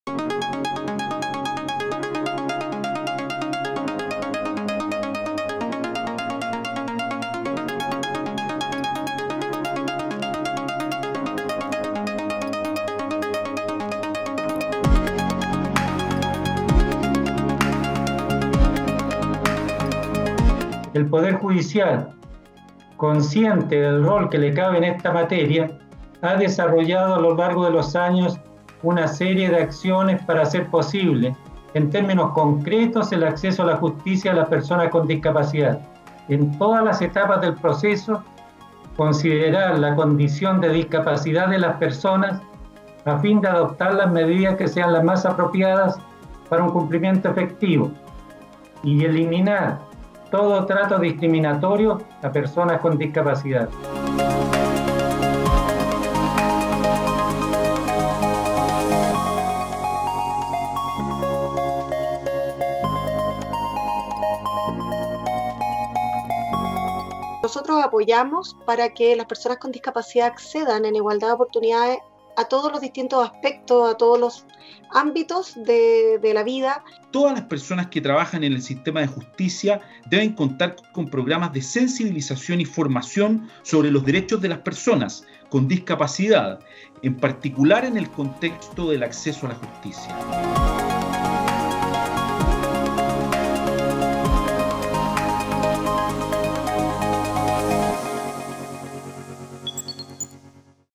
Presidente de la Corte Suprema inauguró seminario sobre DDHH, discapacidad, justicia y sistema penal
El presidente de la Corte Suprema, Guillermo Silva, Guillermo Silva; la directora del Servicio Nacional de la Discapacidad (Senadis), María Ximena Rivas; y el decano de la Facultad de Derecho y Humanidades de Universidad Central de Chile, Emilio Oñate, inauguraron hoy –martes 22 de septiembre- el IV Curso Internacional de Derechos Humanos y Discapacidad en el Ámbito de la Justicia Penal.